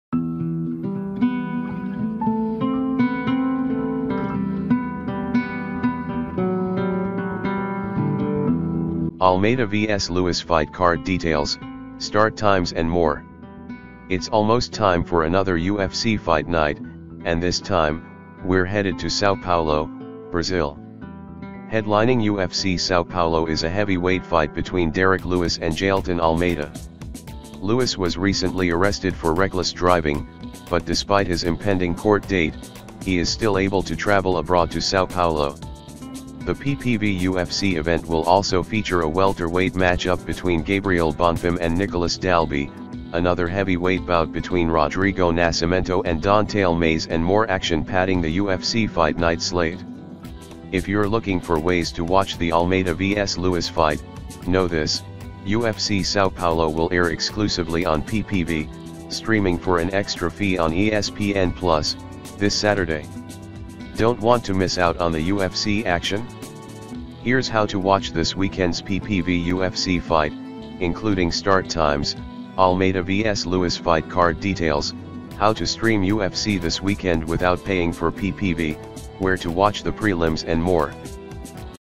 🥊Almeida vs Lewis fight card details, start times and more 🥊
It's almost time for another UFC Fight Night, and this time, we're headed to Sao Paulo, Brazil. Headlining UFC Sao Paulo is a heavyweight fight between Derrick Lewis and Jailton Almeida. Lewis was recently arrested for reckless driving, but despite his impending court date, he is still able to travel abroad to Sao Paulo. The PPV UFC event will also feature a welterweight match up between Gabriel Bonfim and Nicolas Dalby, another heavyweight bout between Rodrigo Nascimento and Don'tale Mayes and more action padding the UFC Fight Night slate.